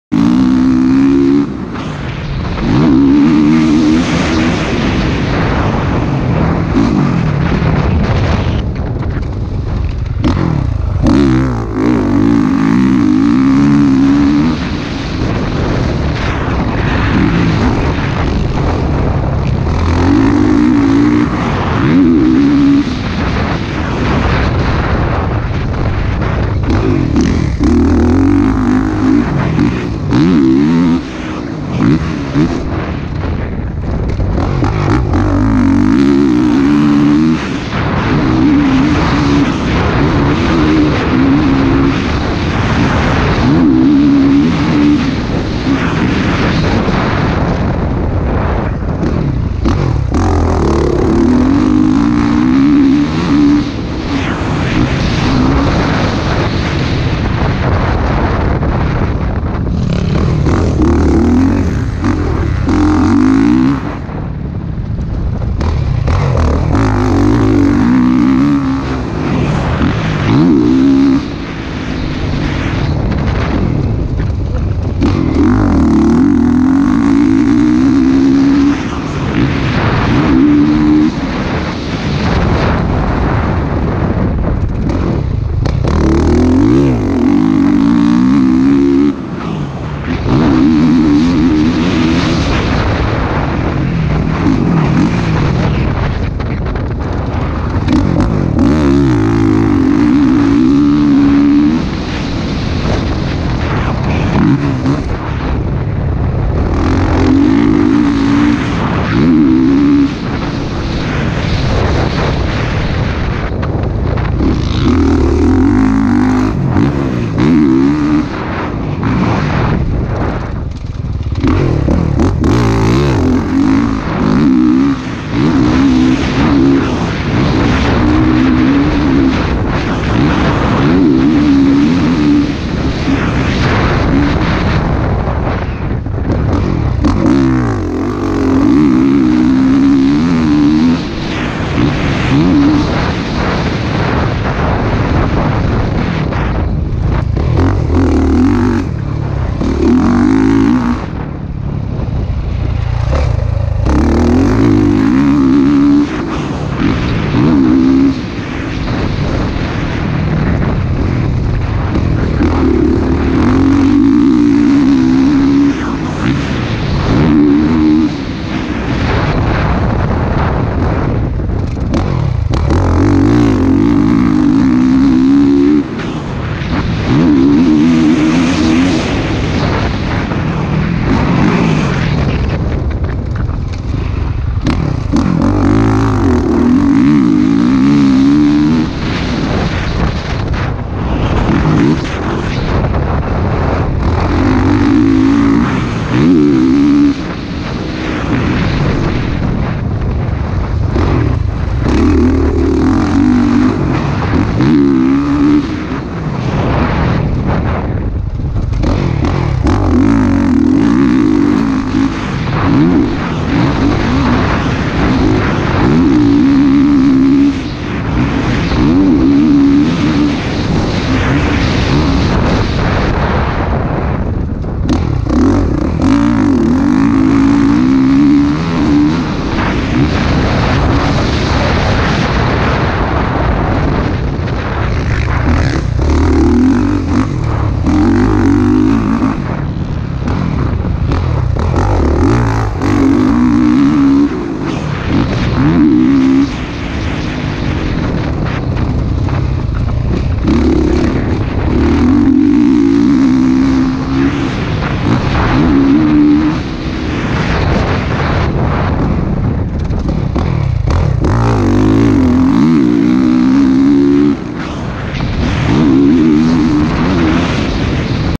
2018 Crf 450r- Raw GoPro- Boise Idaho- Summer Track
Couple hot laps at the local motocross summer track in Boise Idaho!!